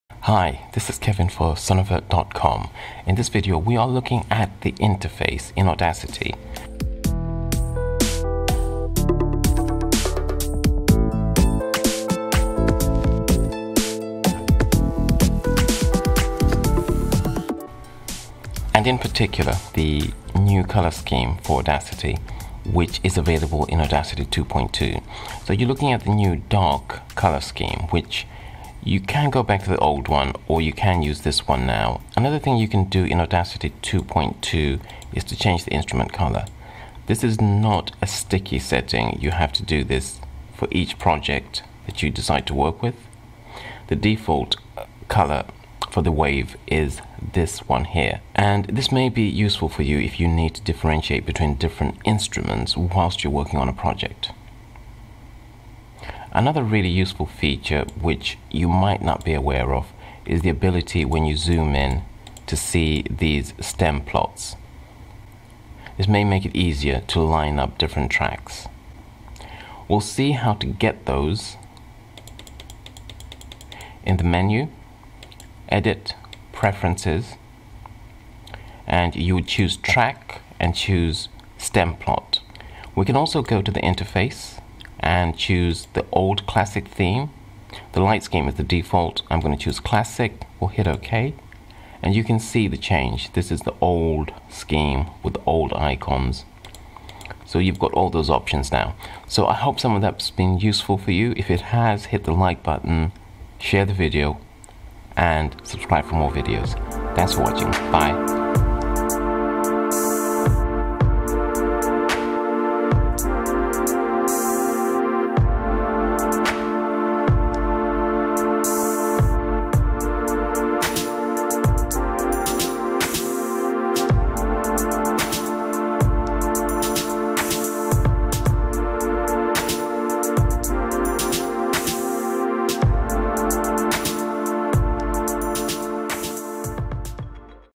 Going Dark: New Audacity Interface Theme and Waveform Colours (Audacity 2.20)
In this video we look at how Audacity can be modified with a new interface dark theme and with new colour options.

As well as dark and light themes, there is the return of the classical theme and a high contrast set up for the visually impaire.

There is also a view that allows the possibility of viewing waveforms much more clearly when zoomed in which will be useful when aligning tracks with millisecond precision.